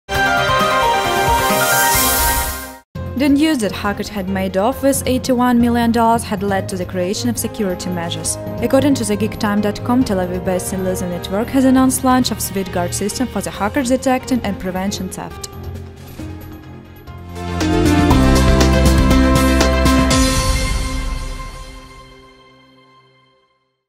KCN News: SWIFT Guard for the hackers detecting and preventing theft
The news that hackers had made off with $81 million had led to the creation of security measures. According to the Geektime.com, Tel Aviv-based illusive networks has announced launch of SWIFT Guard - system for the hackers detecting and preventing theft.